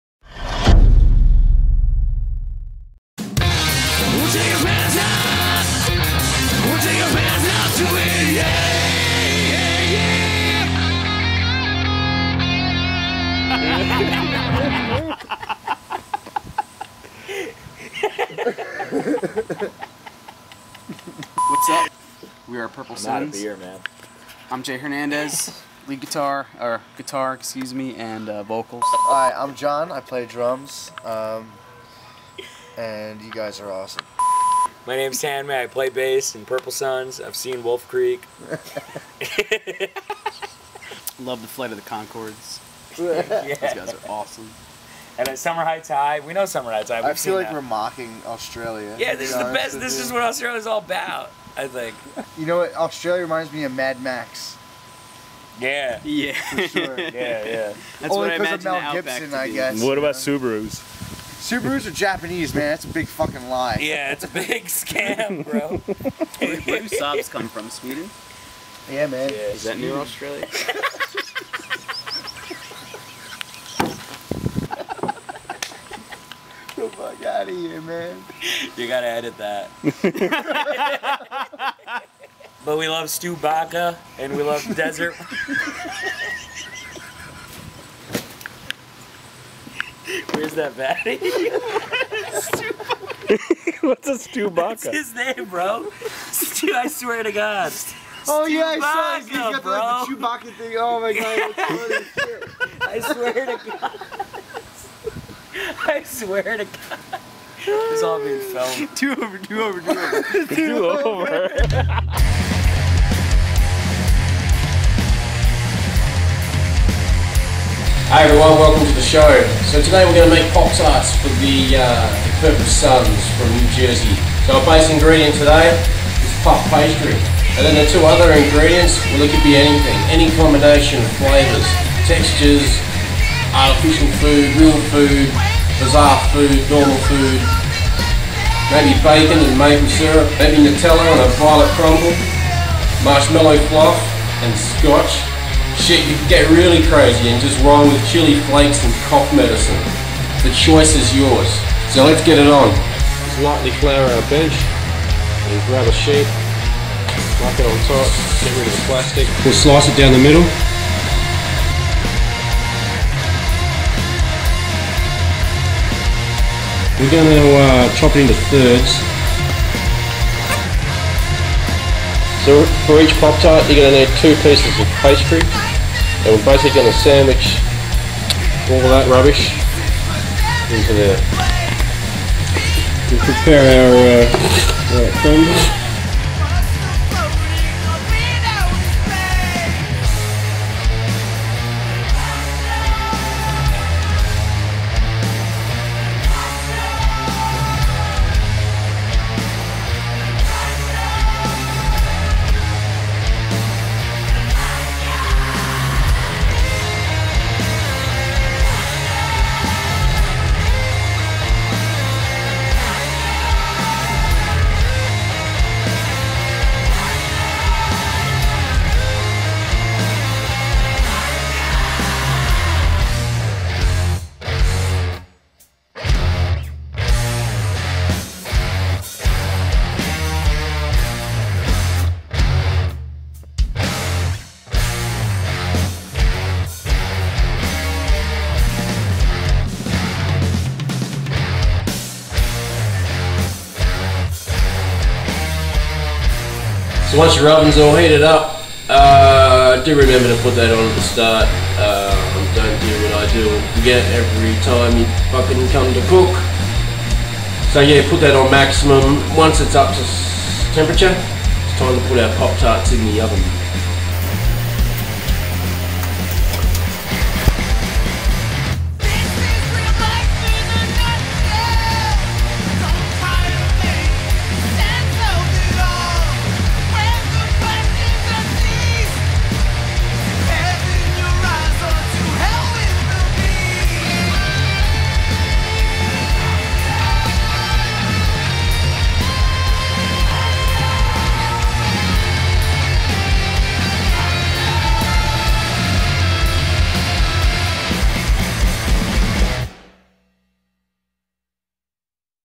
How to make Pop Tarts [DISCO'S KITCHEN] Purple Suns
Experimental 3 ingredient cooking.

If you or your band would like to appear on the menu, hit me up in the facebook link above :) Stay frosty.